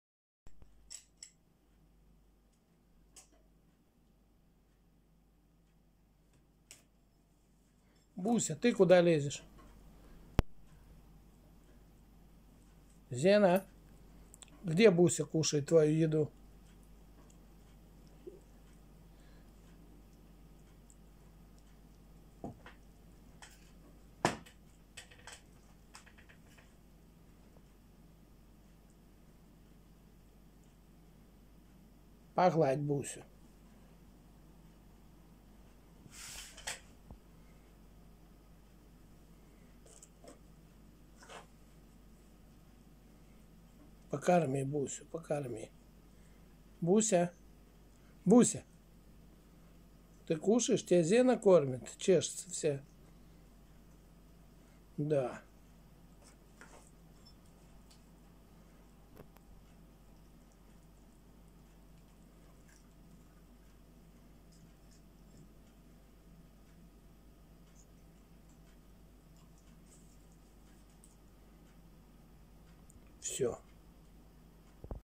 Джек Рассел Терьер Зена кормит кошку Брусничку!!
У детей обед!